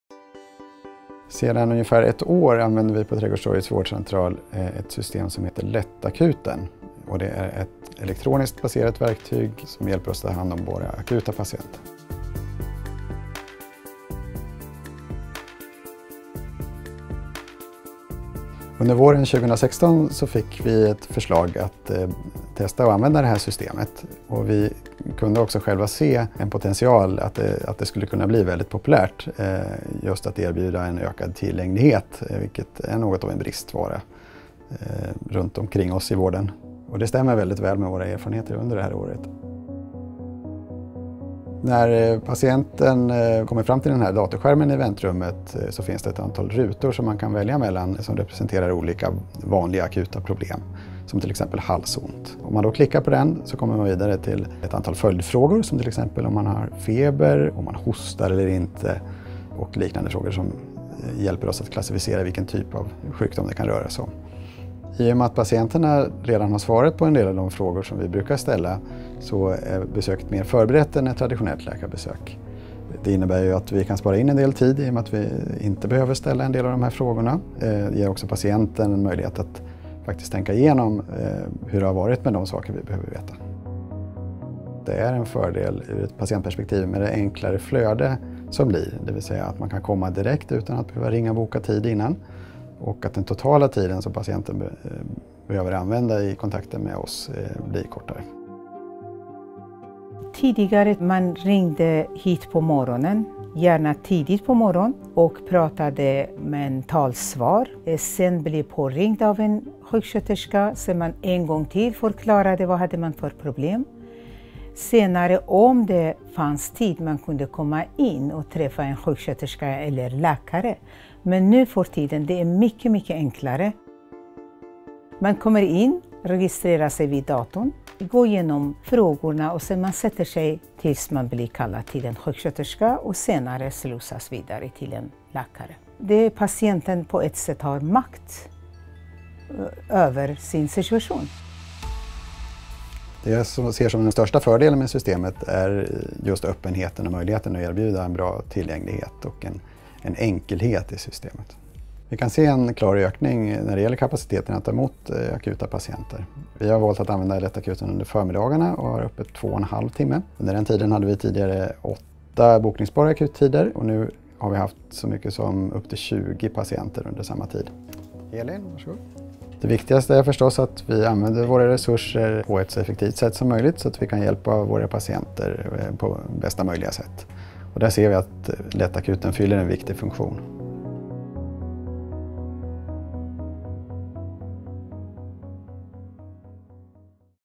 Rätt vård snabbare
Med hjälp av Lättakuten har Trädgårdstorgets vårdcentral ökat tillgängligheten för patienterna.